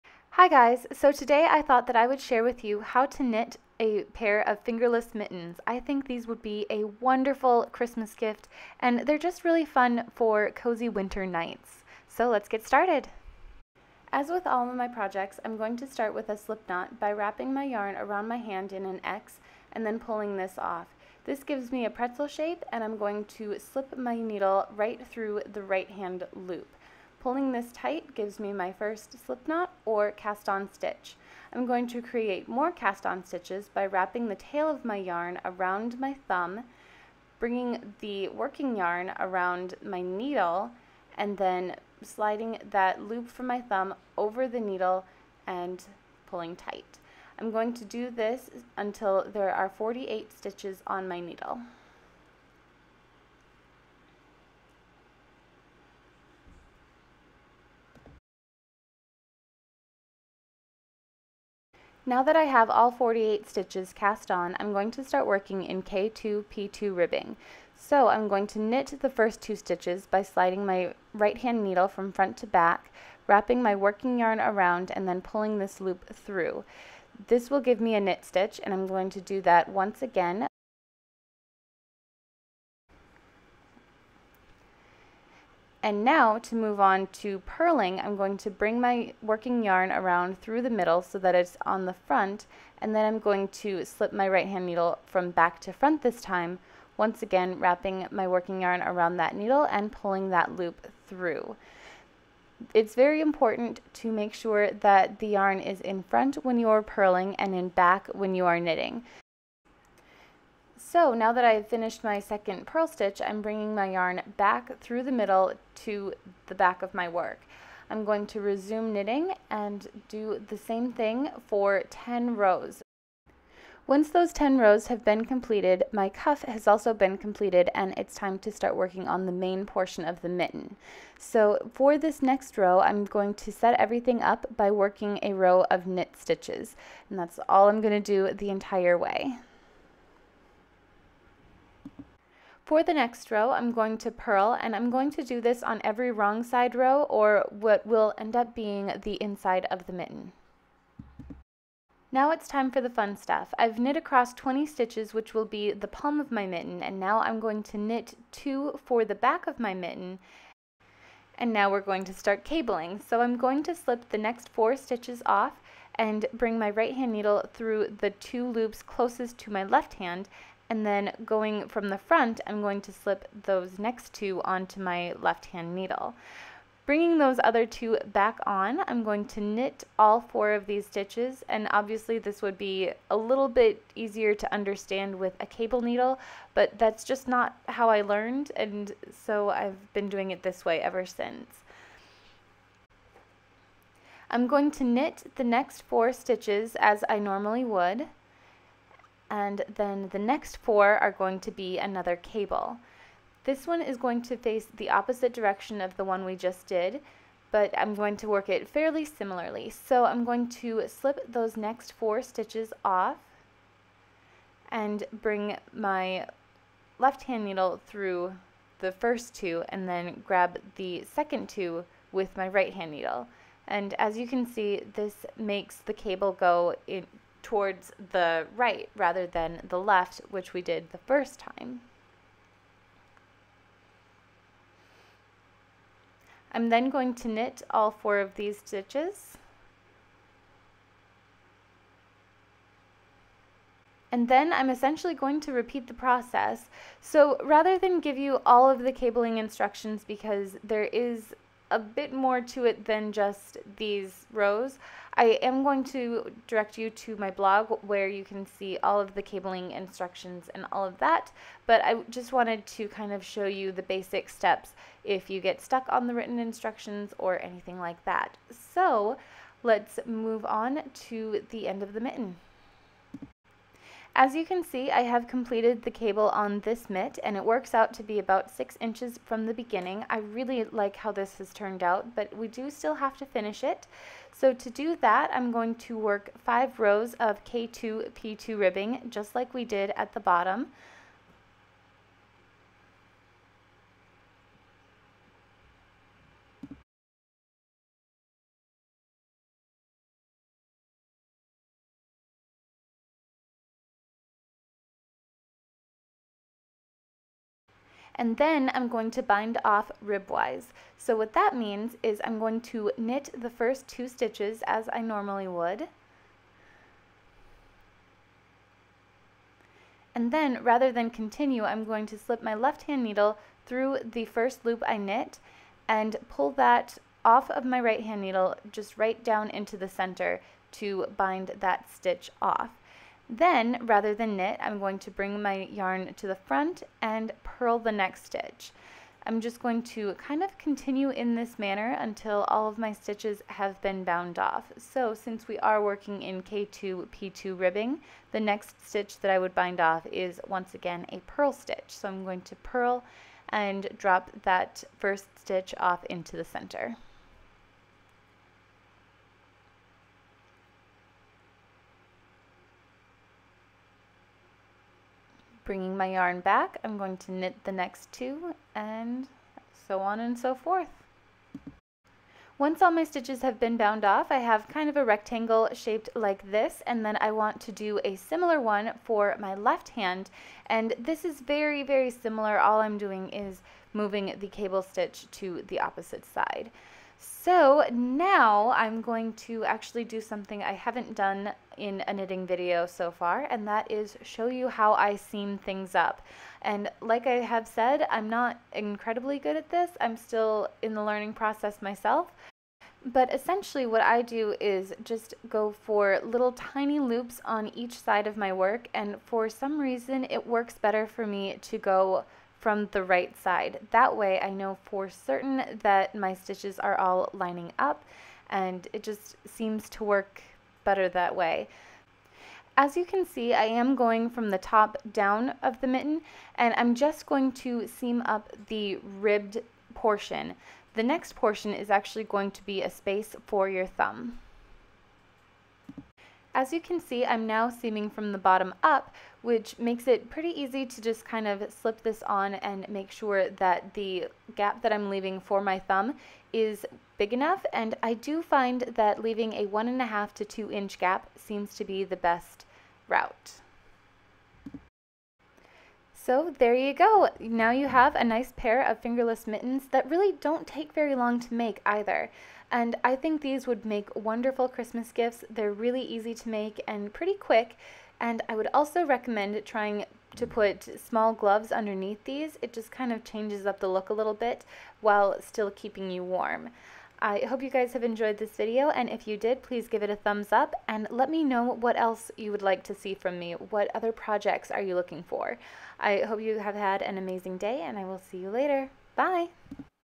How to Knit | Fingerless Mittens
I'm doing another requested knitting video for you guys today!  Honestly, nothing makes me happier than putting together something you guys have asked for, so keep the requests coming!  I am absolutely in love with these fingerless mittens, and I'm actually working on a little hat to go along with them...maybe I'll put the pattern for that on my blog when I get it done! ;)

See what's new on my blog!

Everything shown was purchased by me, and all opinions are my own.